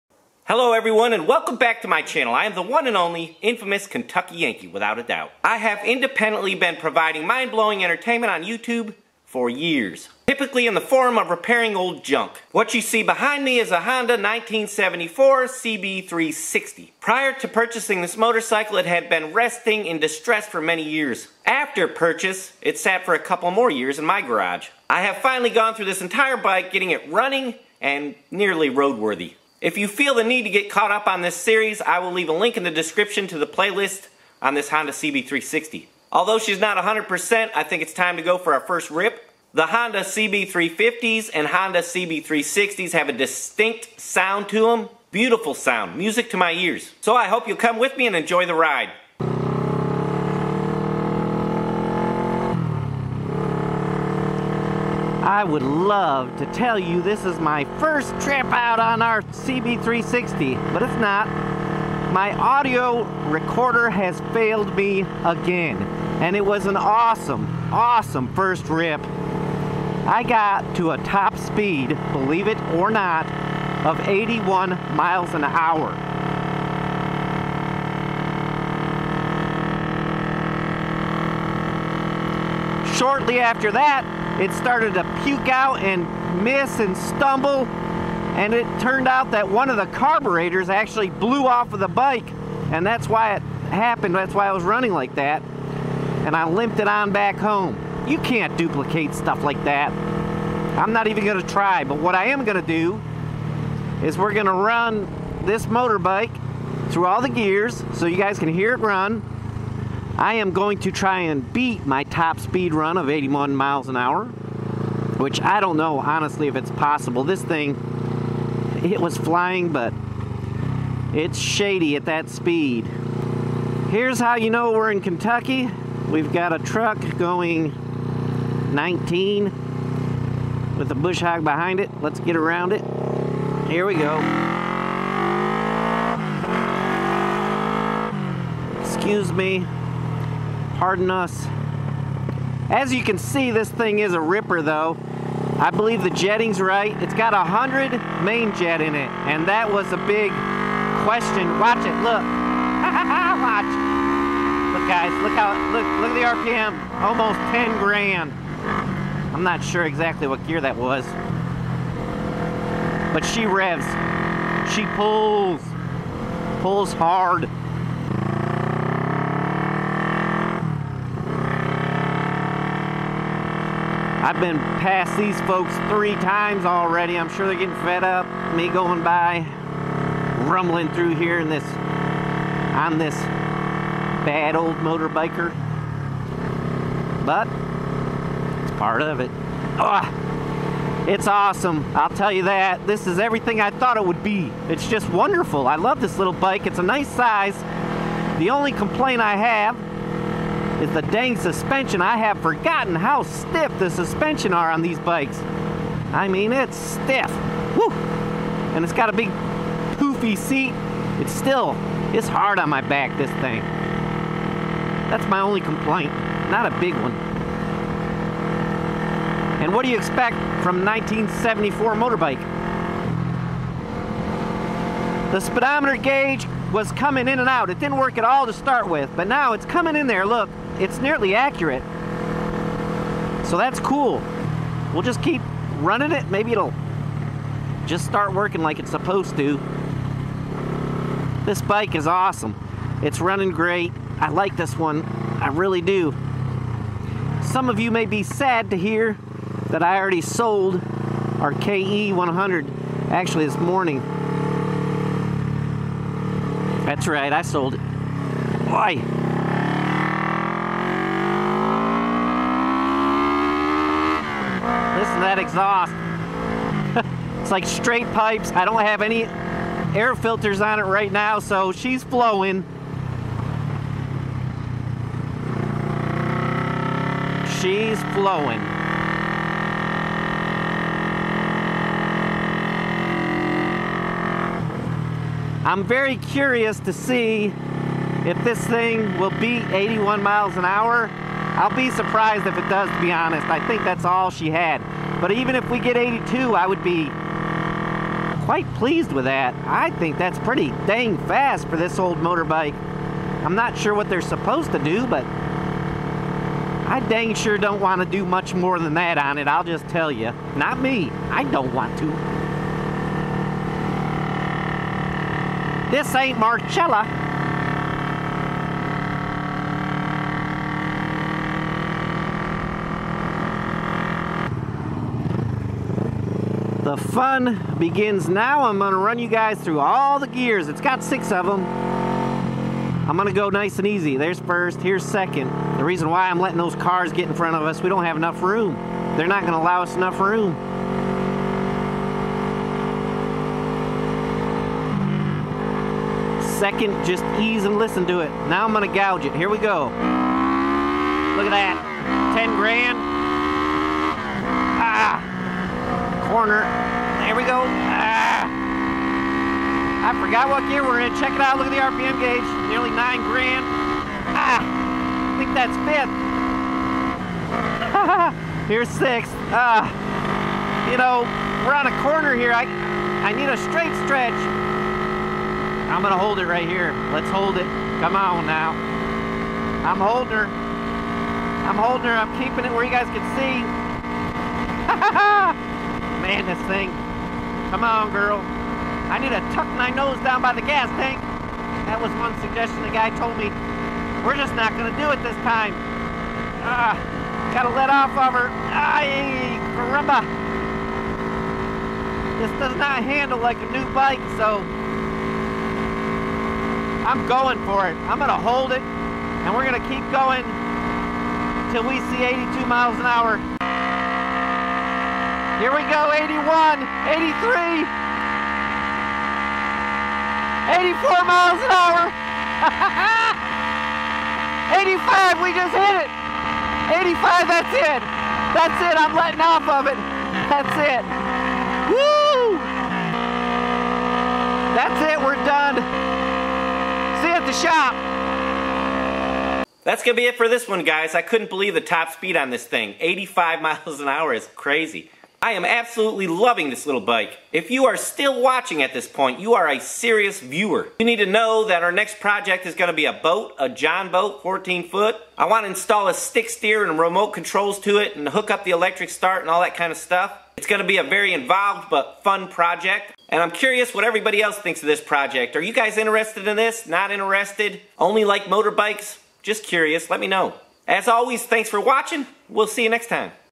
Honda CB360 Short RIDE and TOP SPEED RUN !!
First time on the road with my 1974 Honda CB360, there is no other bike out there that sounds exactly like a Honda twin, and I love it! After going for a short ride I pulled back on the throttle, the acceleration was so great it pinned my ears back! I wanted to see what the top speed would be on this bike right out of the shop after sitting for many years. I was shocked at how fast this little bike went, I think you will be too!